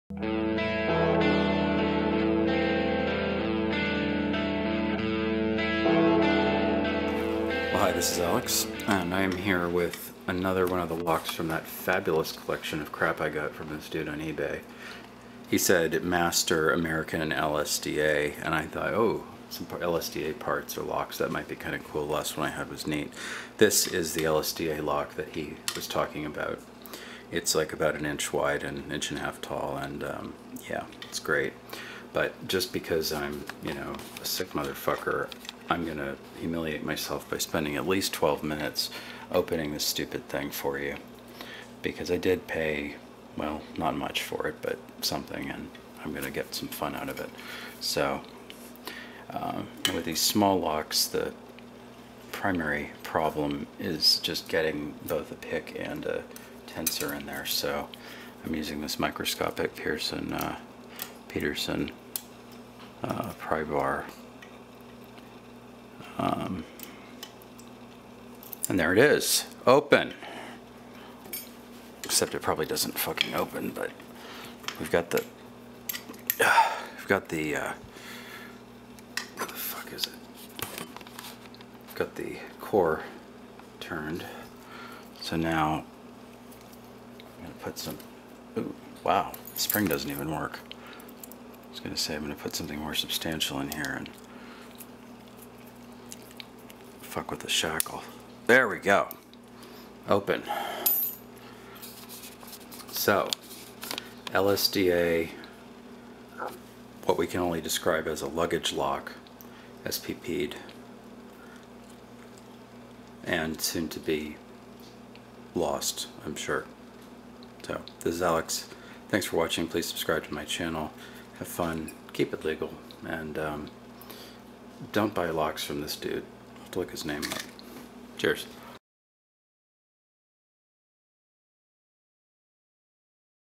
(121) eBag 'o Rust: Part 2: The "LSDA" Padlock -- Surely you jest
So the eBay seller said that this was a lot of American, Master, and LSDA padlocks and parts.

In this installment, we learn exactly what he meant by the LSDA bit.